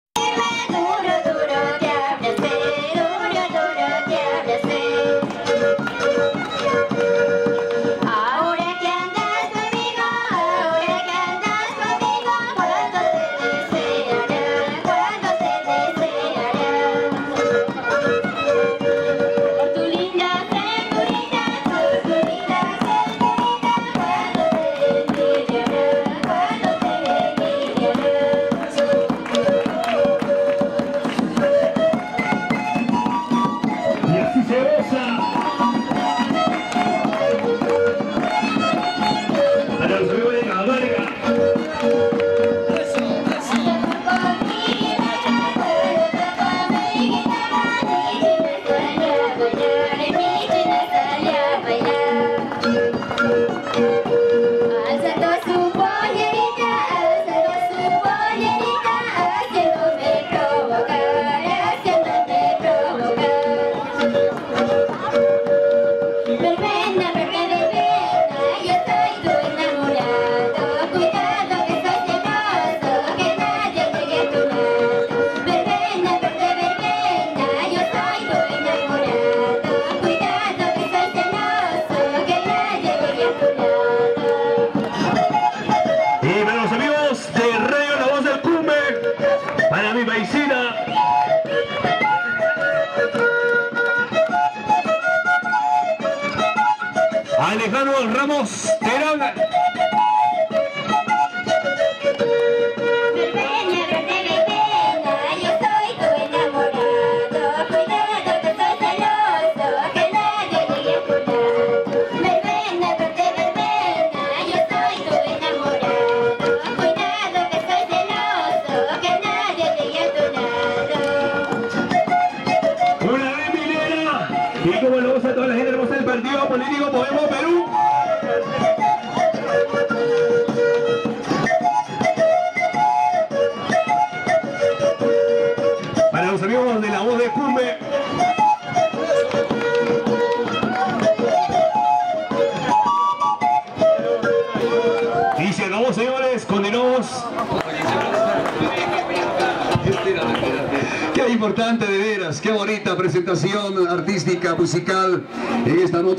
CONTUMAZA 2018 : CANTANTES EN MITIN DE ALAN ANGULO
Jovenes cantantes folcloricas de la provincia de contumaza, deleitando al publico presente con hermosas melodias. Felicidades.